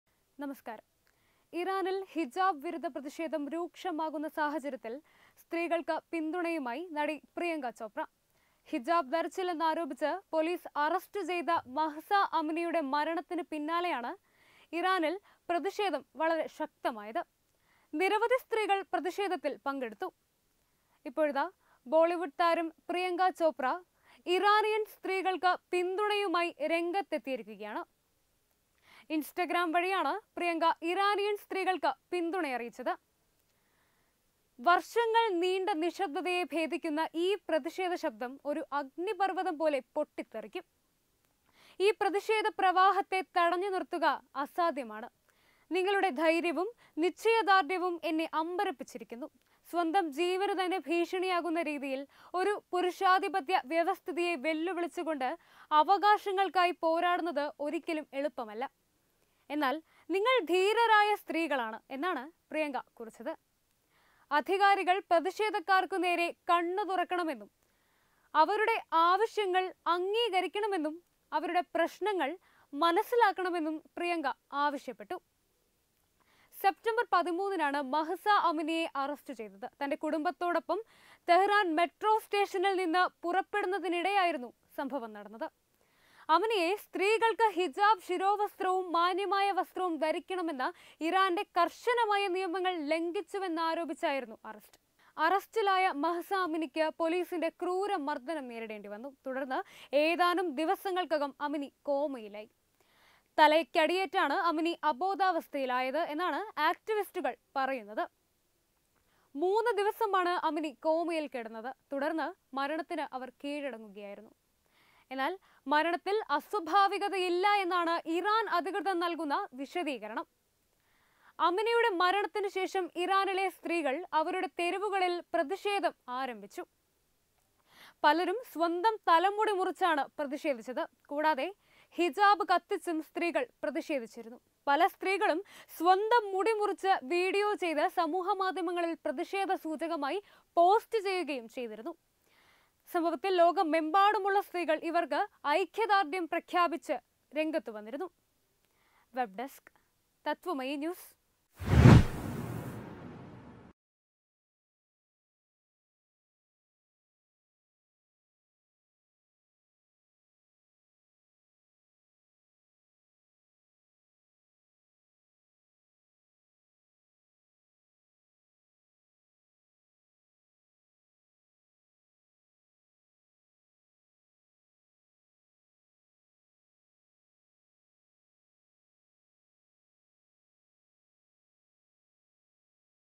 ഇനിയും ഇത് കണ്ടില്ലെന്ന് നടിക്കാനാവില്ലന്ന് പ്രിയങ്ക ചോപ്ര | PRIYANKA CHOPRA
ഇറാനിലെ ഹിജാബിനെതിരെയുള്ള പ്രതിഷേധത്തിൽ പിന്തുണ അറിയിച്ച് പ്രിയങ്ക ചോപ്രയും | HIJAB CONTROVERSY